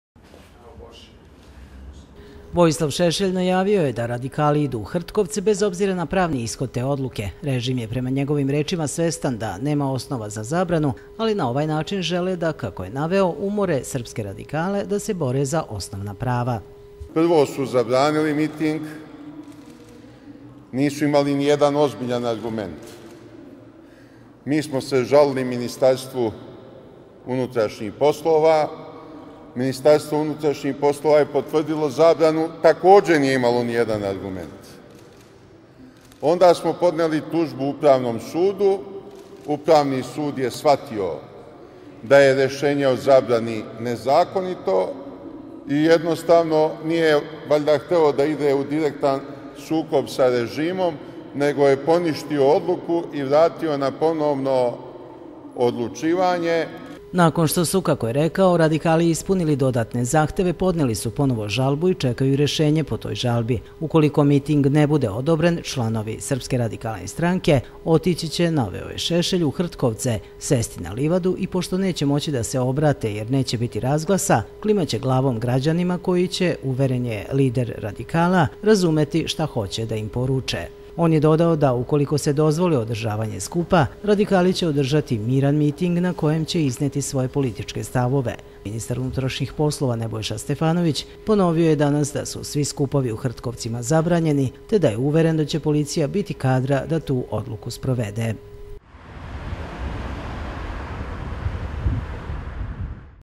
TV KANAL 9, NOVI SAD: ŠEŠELJ NE ODUSTAJE OD HRTKOVACA
Predsednik Srpske radikalne stranke Vojislav Šešelj rekao je danas da će radikali održati skup u Hrtkovcima u nedelju, bez obzira i na drugu zabranu tog događaja. Ministar unutrašnjih poslova i potpredsednik vlade Nebojša Stefanović ponovio je da su svi skupovi u Hrtkovcima zabranjeni i da je uveren da će policija biti sposobna sprovede tu odluku.